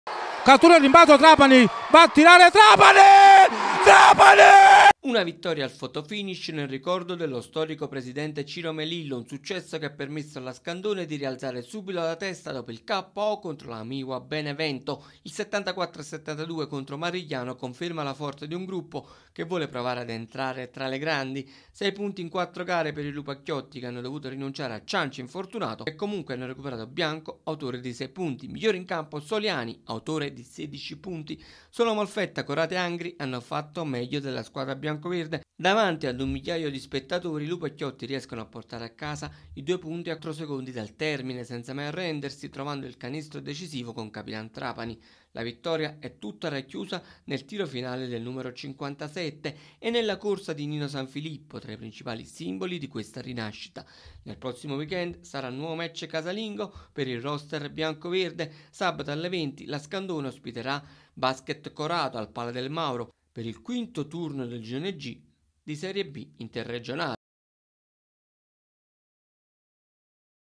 Trapani al fotofinish, la Scandone vince nel ricordo di Melillo
Terza vittoria in quattro gare per i ragazzi di Sanfilippo. 74 a 72 il finale.